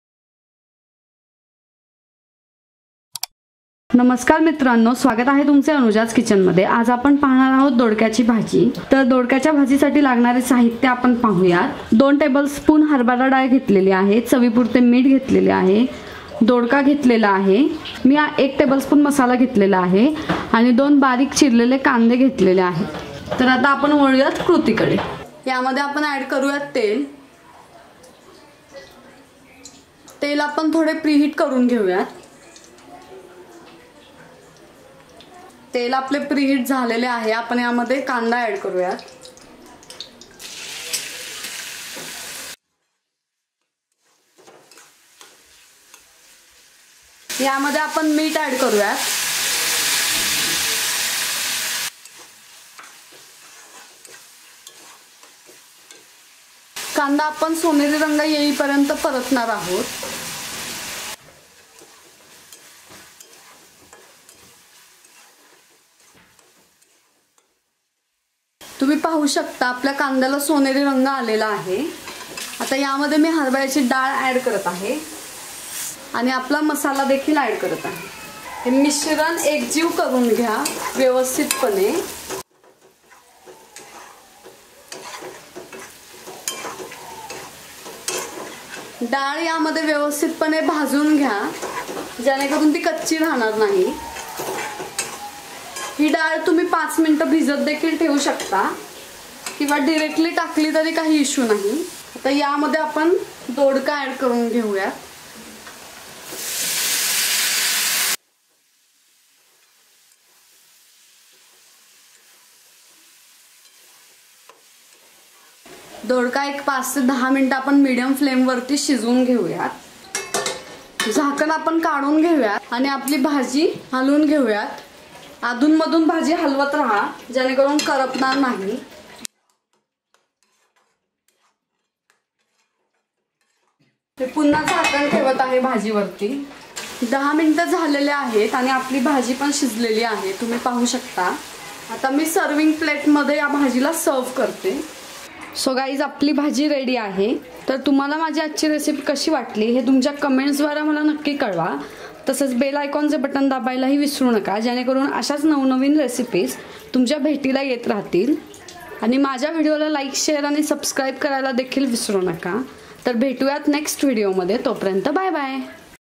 Dodkyachi Bhaji/दोडक्याची भाजी/गावरान पद्धतीचा झणझणीत दोडका/Dodka Bhaji/दोडक्याच्या भाजीची रेसिपी
नमस्कार मित्रांनो ,
आजच्या या विडिओ मध्ये आपण बघणार आहोत दोडक्याची भाजी कशी बनवायची
तेही 2:46 मिनिटामध्ये  …..

दोडक्याची भाजी बनवण्यासाठी लागणारे साहित्य …
२ चमचे हरभरा डाळ
मीठ
दोडका
१ चमचा मसाला
२ कांदे